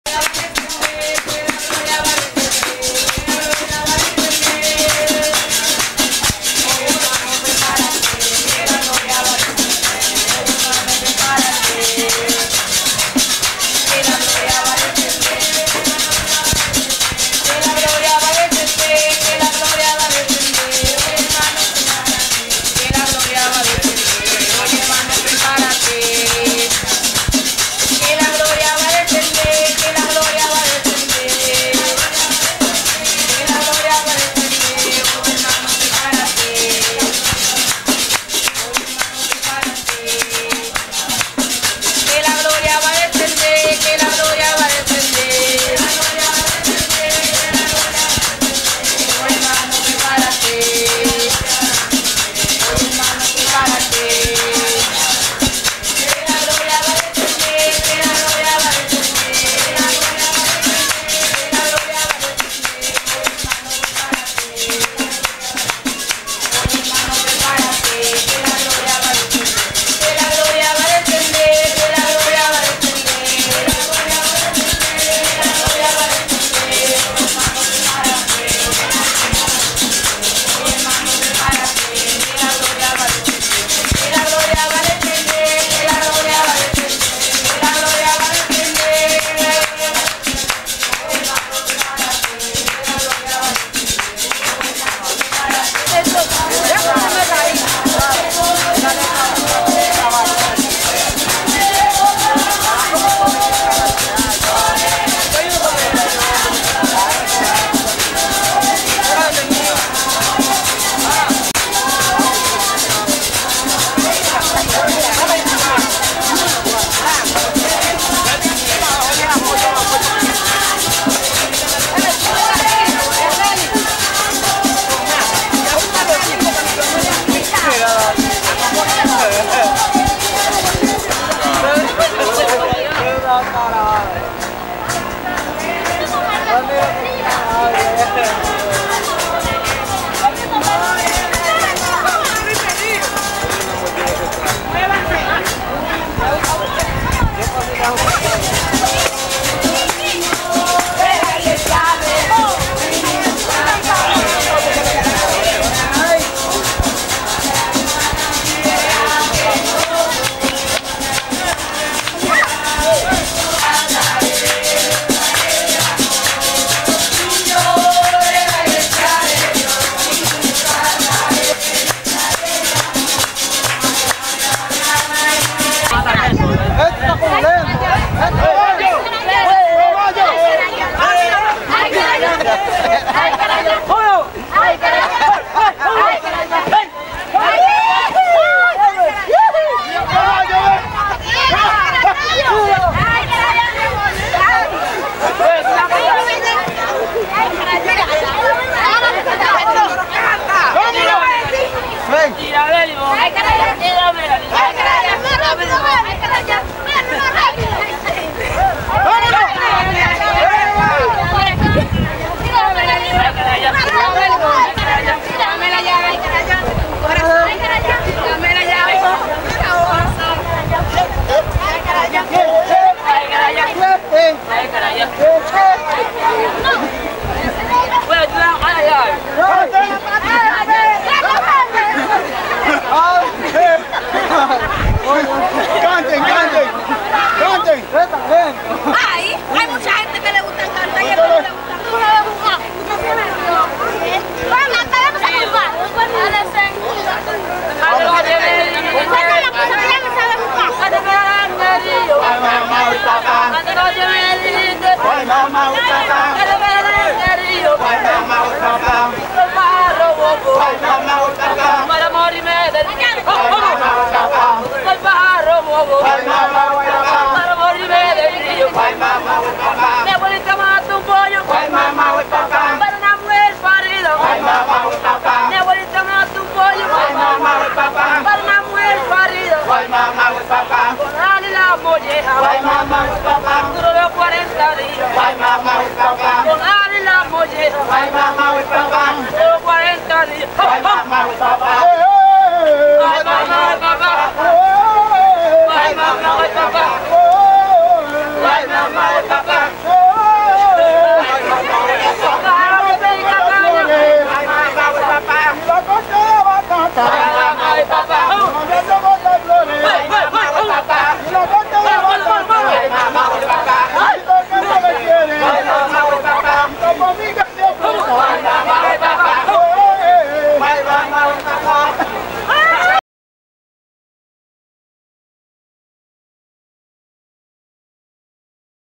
Fiestas a la cosecha o Harvest en iglesia evangélica de Monte Rojo, en Samaná.
Interior iglesia, joven tocando marimba. Adultos y jóvenes jugando los juegos denominados Karaya.

Cámara: Soraya Aracena
Monte Rojo, Samaná, República Dominicana
1999